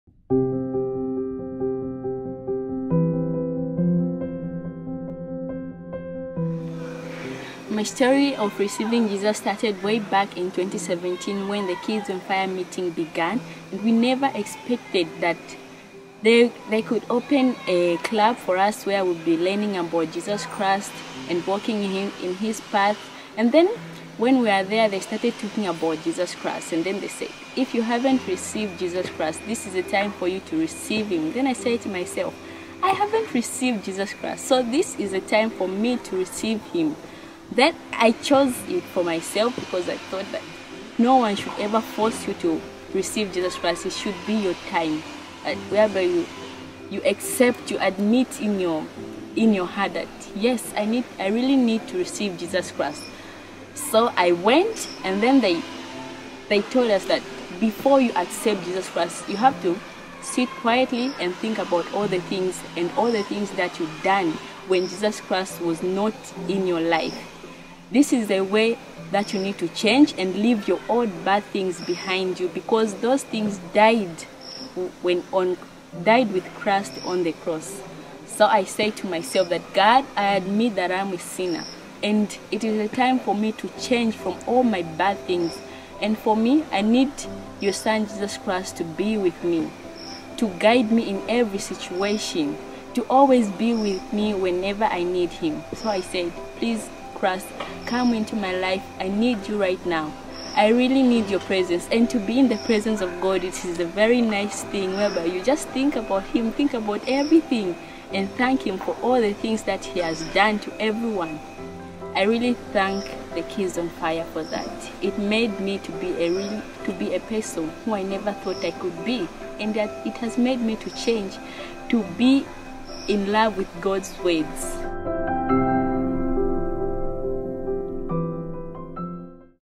Kids on Fire Testimony
Martha shares about how she came to know Jesus and how her life has changed because of knowing Him!